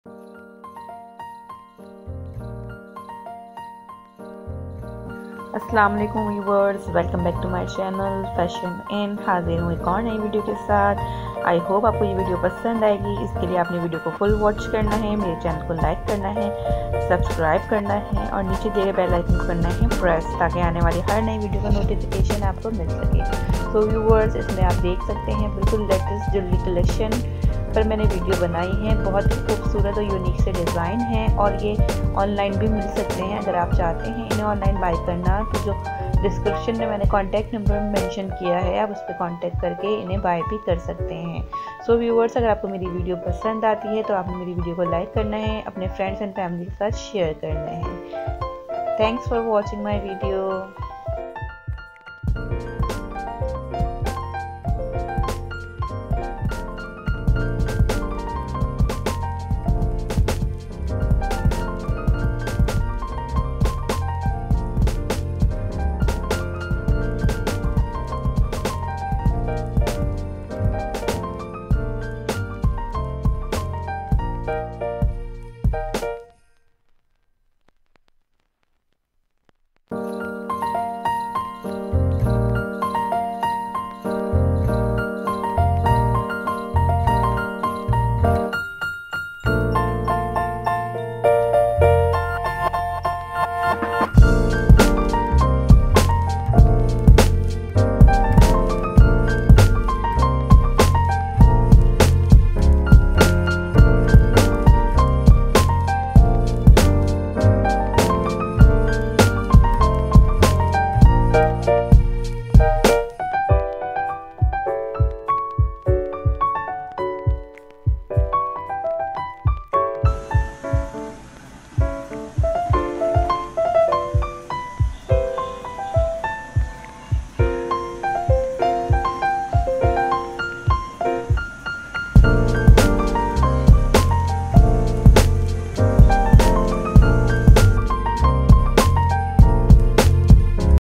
Beautiful jewellery collection 2021/unique jewellery designs#
Asalamualikum,
viewers aje ki is video me jewellery k bilkul new designs hai ager ap buy Karna chaty han to is what's app number par contact kark buy kar sakty han